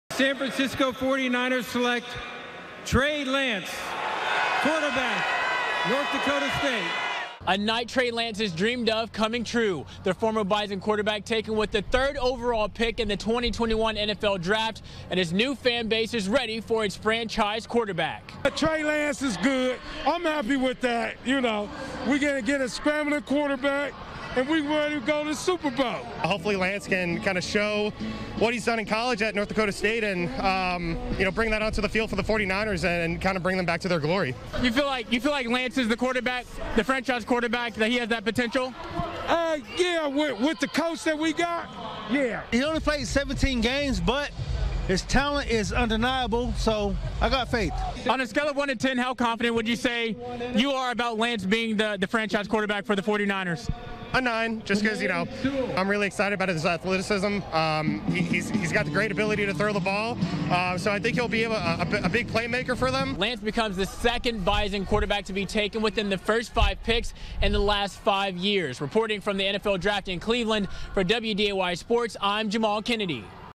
49ers fans react to Trey Lance draft selection
Shortly following Trey Lance's selection in the NFL Draft, I walked around just outside FirstEnergy Stadium in Cleveland and talked to Niners fans about their future QB.